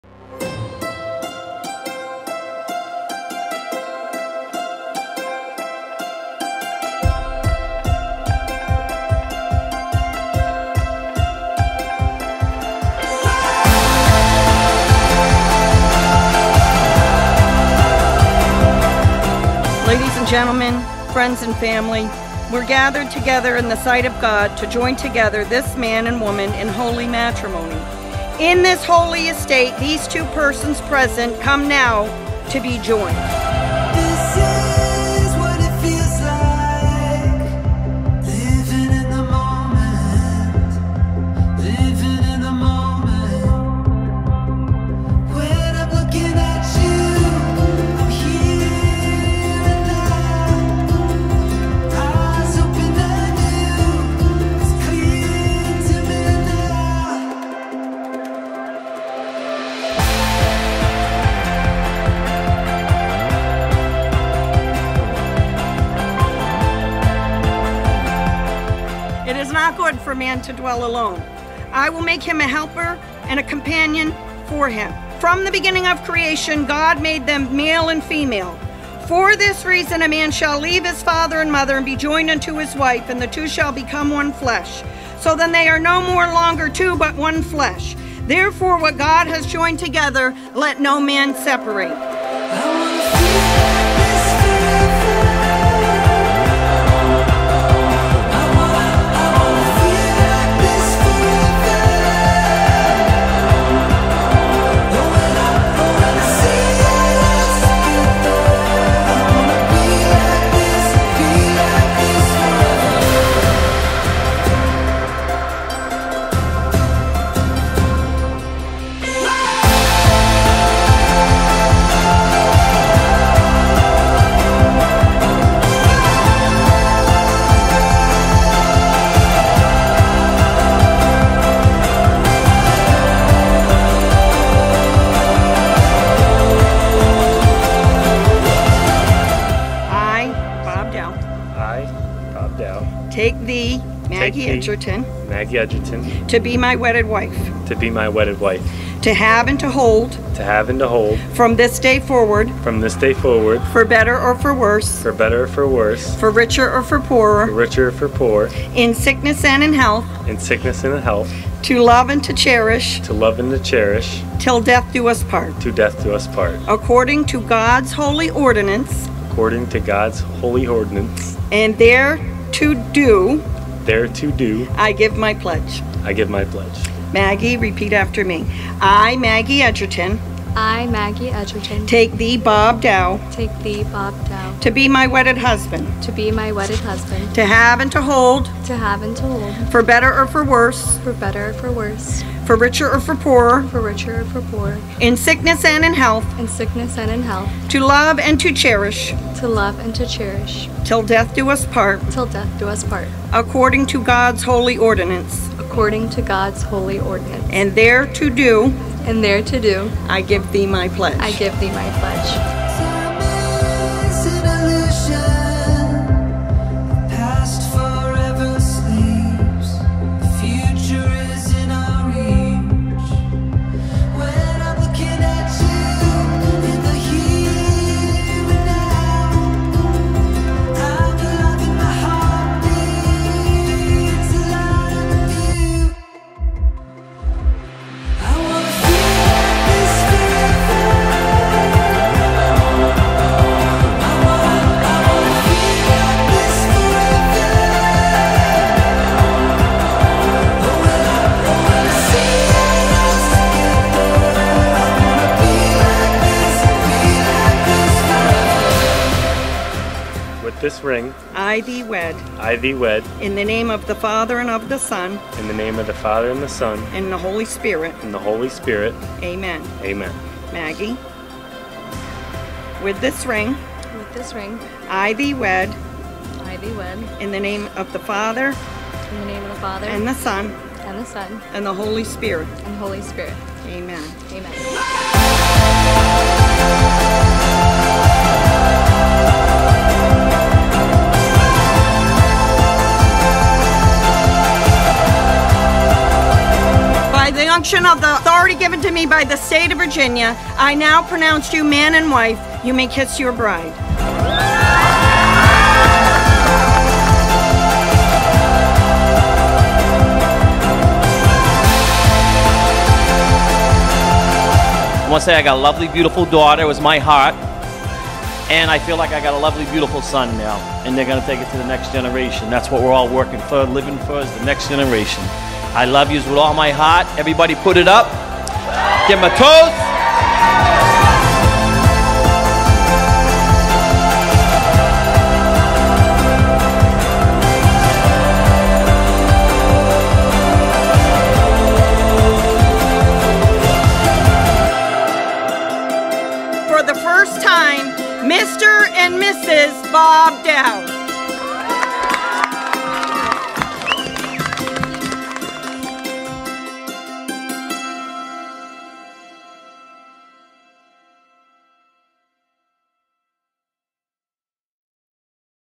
Maggie & Bob's Wedding (FULL VIDEO) at Fort Lauderdale Marriott Harbor Beach Resort & Spa Florida
Venue at Fort Lauderdale Marriott Harbor Beach Resort & Spa